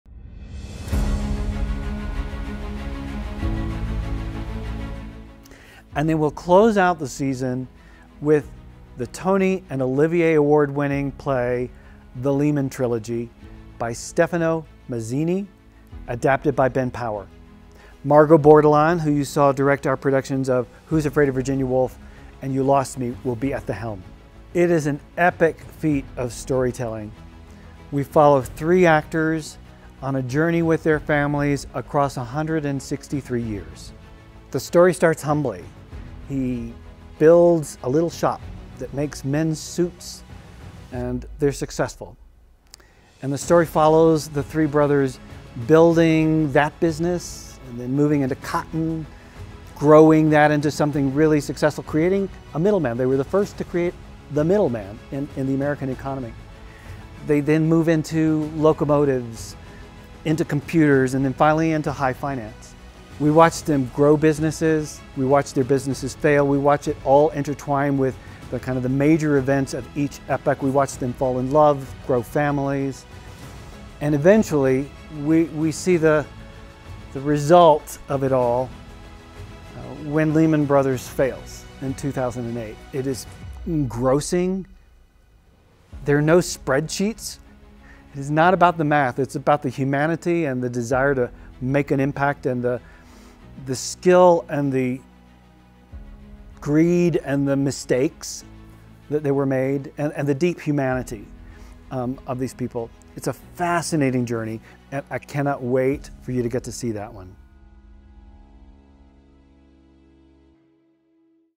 The Lehman Trilogy – The 2023/2024 Theatre Company Season
Winner of the 2022 Tony Award for Best Play and hailed as “a genuinely epic production” (The New York Times), The Lehman Trilogy follows three brothers, and their descendants, as they navigate fire, flood, war, and panic to build a financial behemoth that changed America.

In 1847, the Lehman brothers open a modest clothing shop in Alabama. But they have big dreams. They evolve as new opportunities arise. They capitalize on railroads, oil booms, personal computers, and, eventually, capital itself. They become so intertwined with the U.S. government, and in the daily lives of millions of stakeholders, that some begin to believe that Lehman Brothers, the institution, is too big to fail.

This extraordinary feat of storytelling uses only three actors to trace 163 years of family history and business. Until one day, in 2008, when it all comes crashing down…
May 3 – Jun 2, 2024
Kilstrom Theatre
The Lehman Trilogy
By Stefano Massini
Adapted by Ben Power
Directed by Margot Bordelon